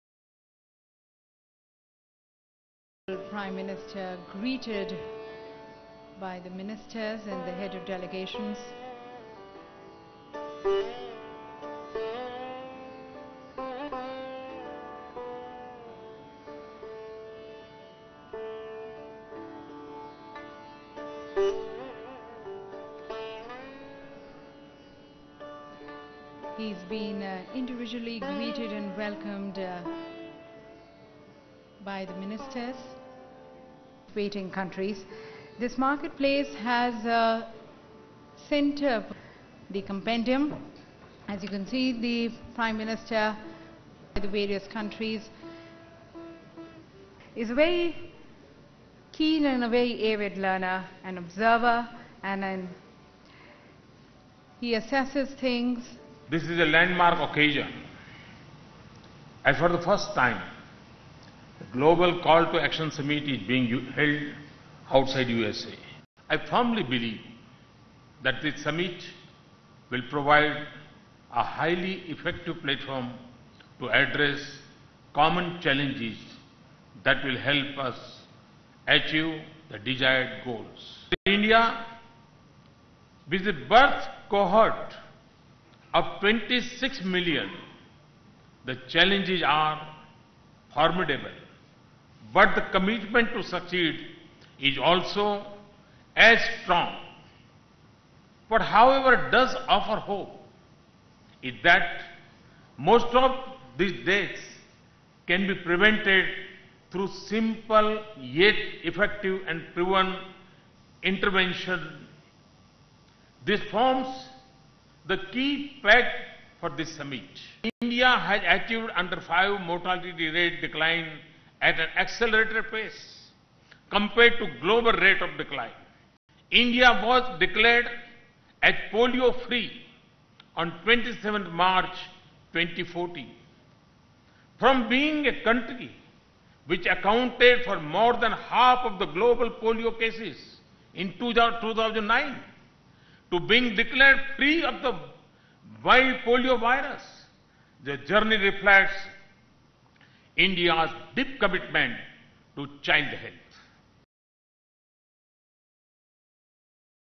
India committed to bolster superior child and maternal healthcare measures: PM Modi (Part - 1)
New Delhi, Aug 27 (ANI): While addressing the gathering at Global Call to Action Summit in national capital Prime Minister Narendra Modi expressed India's commitment to bolster healthcare measure for child and maternal care. Modi also shared his delight of the fact that this is a landmark occasion and for the first time as this summit is being held outside USA. He said this summit is an effective platform to address the common challenges faced by the 24 participant countries. Modi also underscored India's feat of becoming polio free country after being declared as a country accounting for half of the polio cases of the world. He highlighted this transition as the India's perseverance to eradicate polio and is determined to superior healthcare measures. Modi also announced various programs that would help provide momentum in achieving the desired goals in the field of infants and nursing mothers' healthcare. He also spoke of 'Mission Indradhanush' that is aimed at vaccinating all the children to keep them safe from all vaccine preventable diseases.  The Ministry of Health and Family Welfare, Government of India, will co-host the CALL TO ACTION SUMMIT 2015 for ending preventable child and maternal deaths along with Ethiopia's Ministry of Health, USAID, UNICEF. The two-day Leadership Summit will be a confluence of health ministers from 24 priority countries.